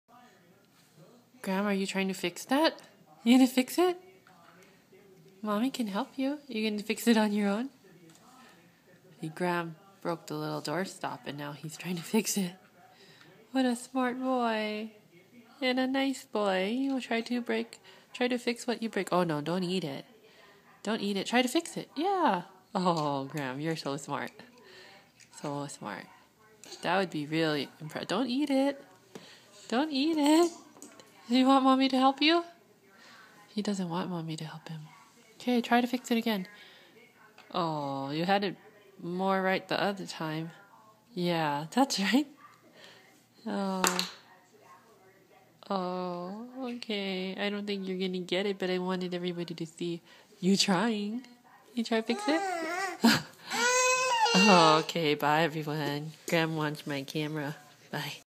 Graham Fixes Doorstop
My little handy man.